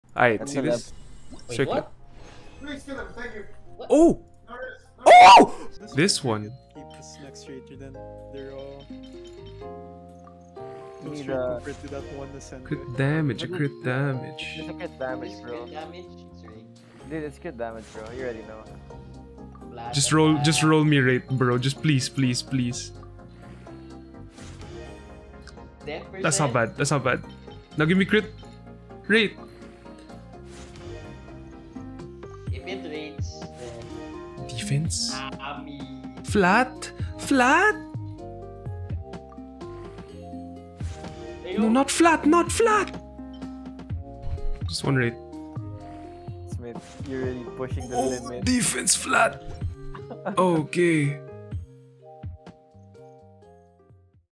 WHEN YOU GET A PERFECT GEO GOBLET
Just when I thought farming for this set was over, after finally getting a 4 pc set. It is what it is